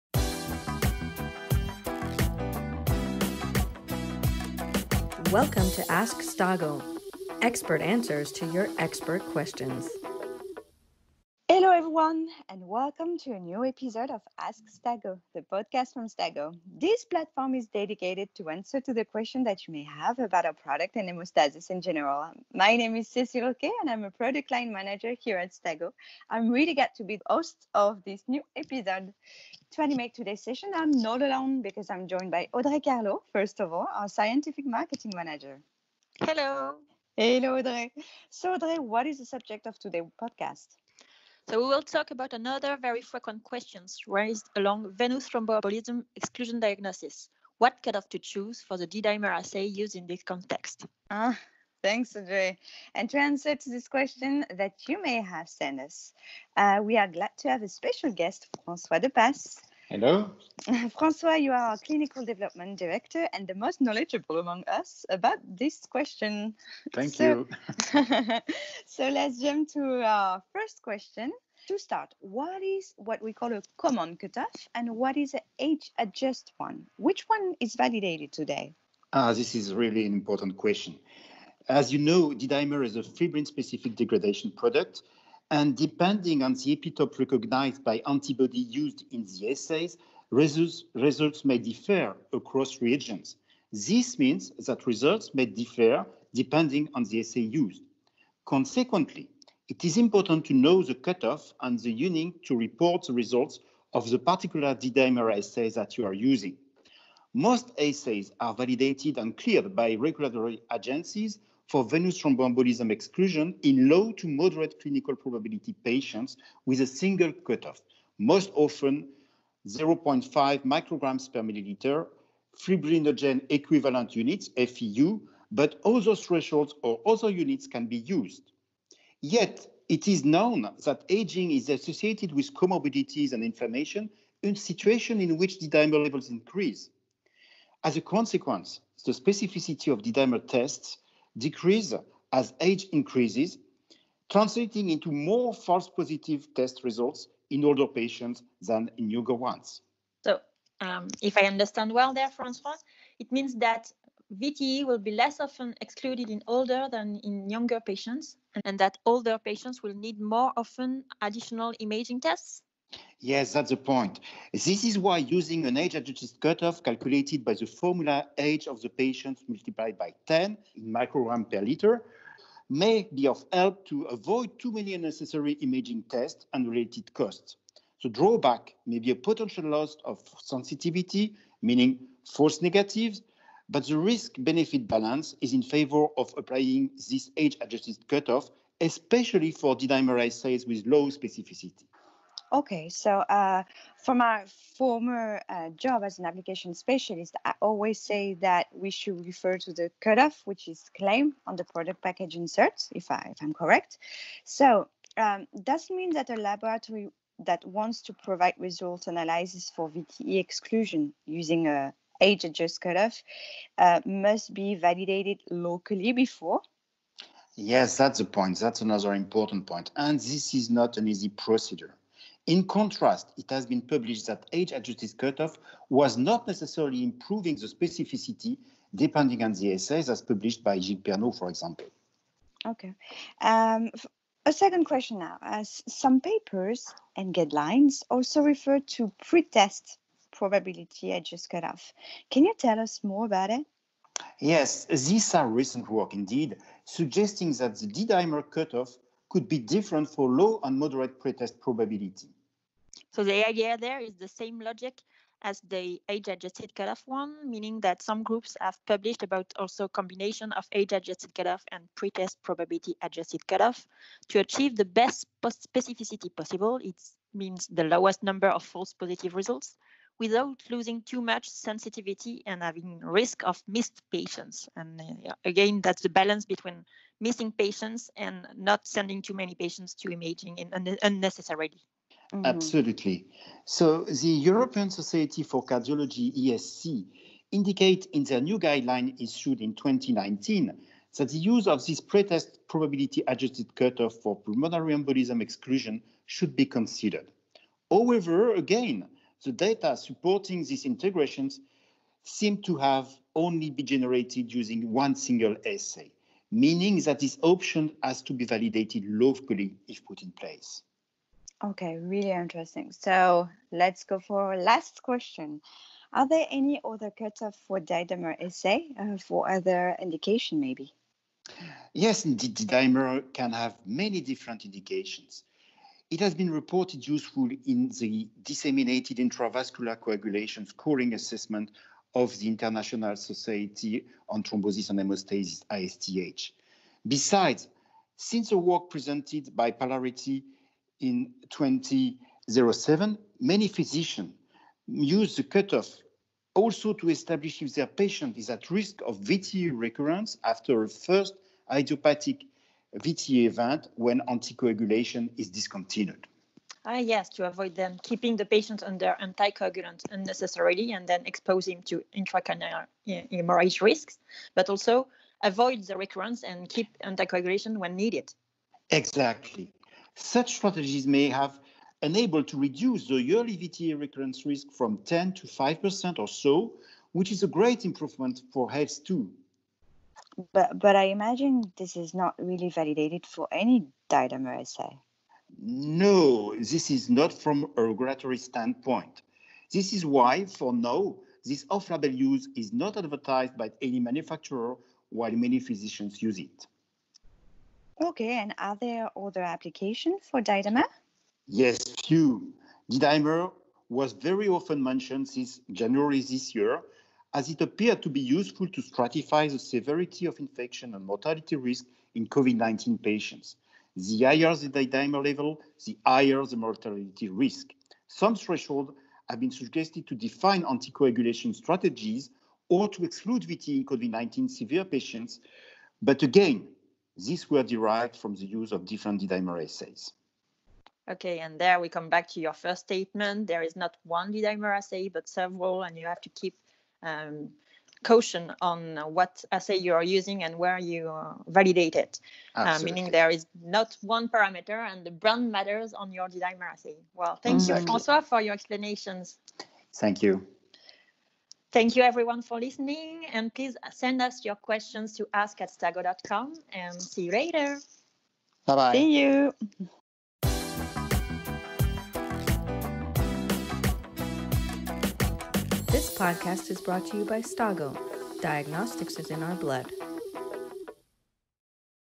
#4 D-Dimer and its cut-offs for VTE exclusion
Presented by Audrey Carlo and Cécile Hourquet // Special guest François Depasse, Clinical Development Director at Stago

Welcome to Ask Stago, the Podcast providing expert answers to your expert questions.
In today’s episode, with François Depasse our Clinical Development Director, we will discuss the selection of a cut-off for D-dimer assay in venous thromboembolism exclusion diagnosis.

Sources:  Pernod G, Maignan M, Marlu R. Questioning the use of an age-adjusted D-dimer threshold to exclude venous thromboembolism: analysis of individual patient data from two diagnostic studies: comment. J Thromb Haemost 2016; 14: 2553–4.

Content is scientific and technical in nature. It is intended as an educational tool for laboratory professionals and topics discussed are not intended as recommendations or as commentary on appropriate clinical practice.